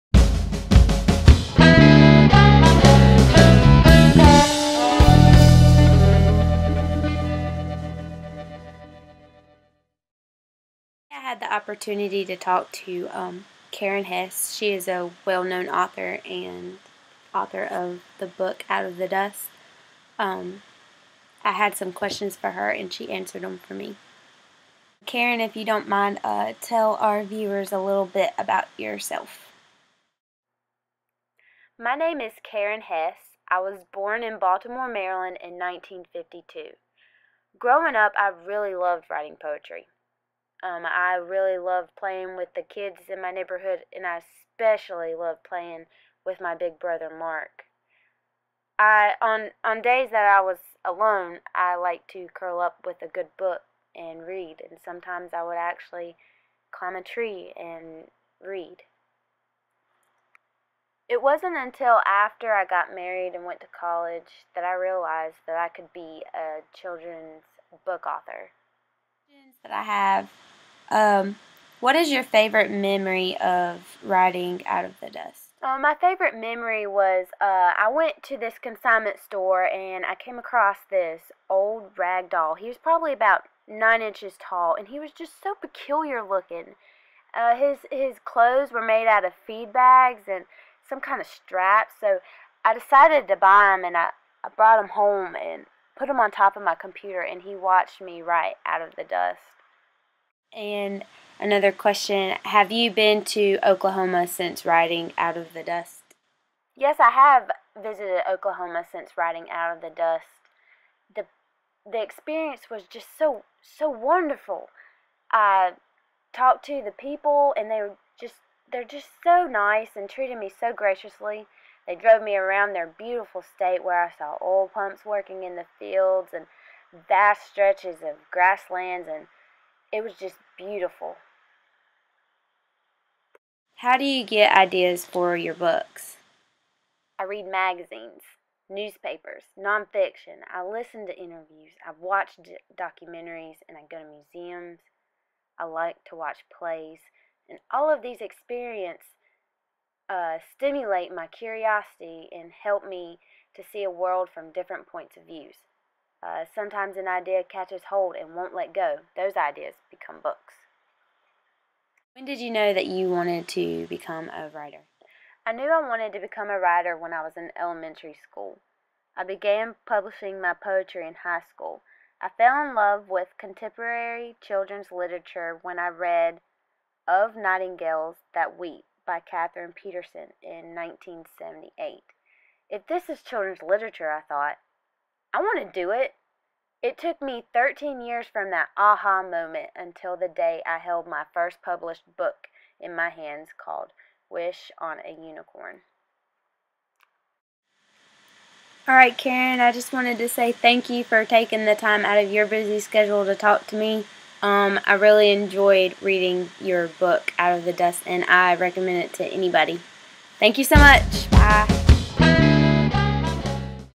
Karen Hesse and Jill Totten Interview
This is a video made for an Elementary Methods in Social Studies class at Catawba College based on the time period and book Out of the Dust by Karen Hesse.

Images shown are not mine, nor do I take credit. Images were taken from Google images and are given credit.